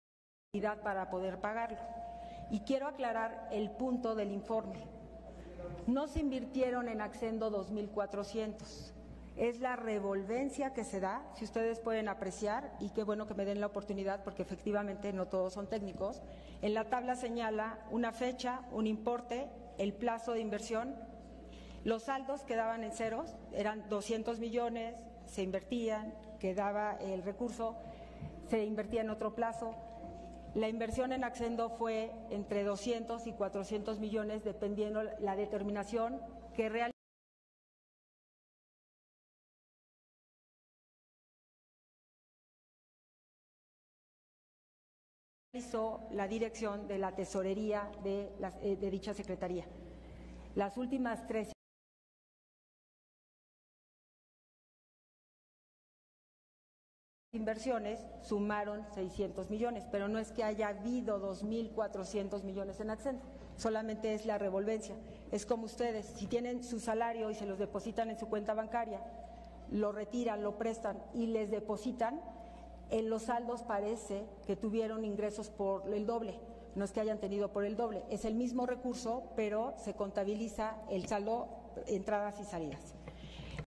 Rechaza Castro Corro que la inversión en Accendo sea Superior a 2 mil 400 millones de pesos
Rechaza Castro Corro que la inversión en Accendo sea Superior a 2 mil 400 millones de pesos, refirió que son "revolventes" y lo aclaró por qué diputados no son “técnicos”.